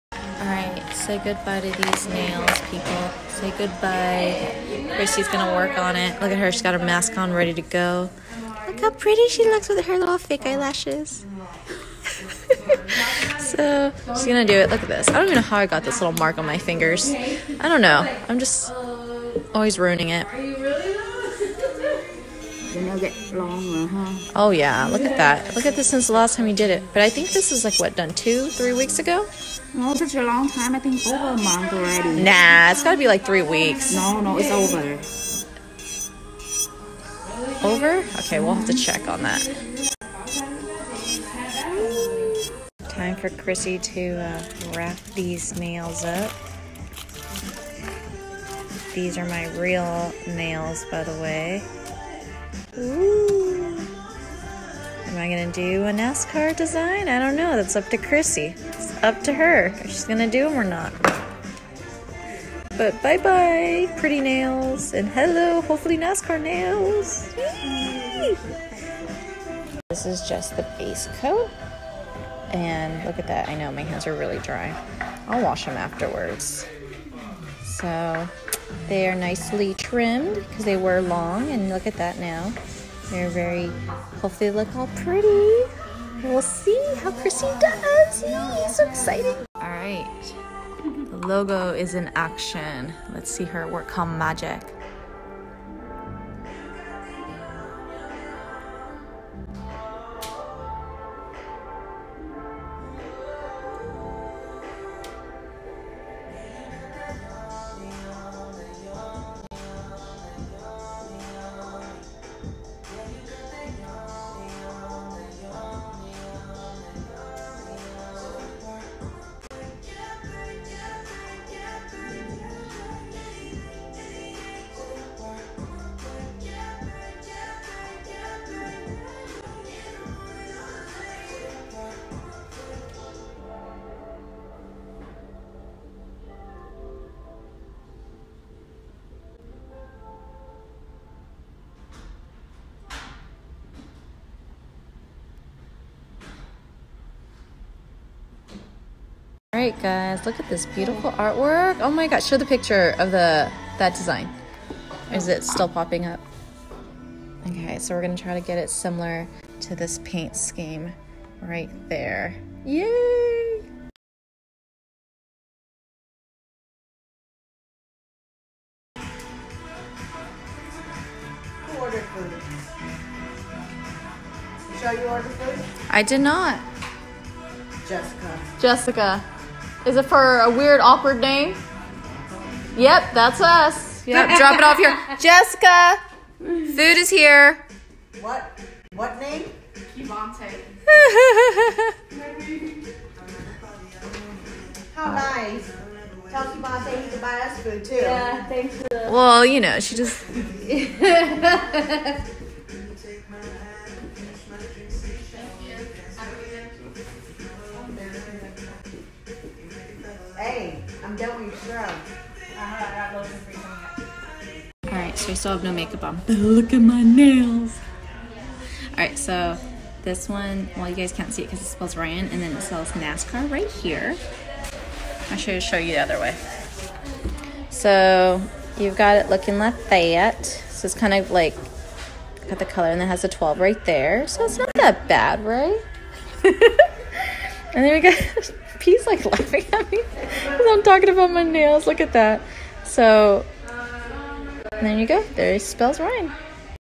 BirthdayNails NASCARNailArt
For my 35th Birthday, as today is my official birthday... figured it was perfect timing for loading this video of me getting my nails done Tuesday to root for Ryan Blaney, my crush ... Since my other loves Dale Jr and Kasey are both retired. Happy Birthday Me 2.16.20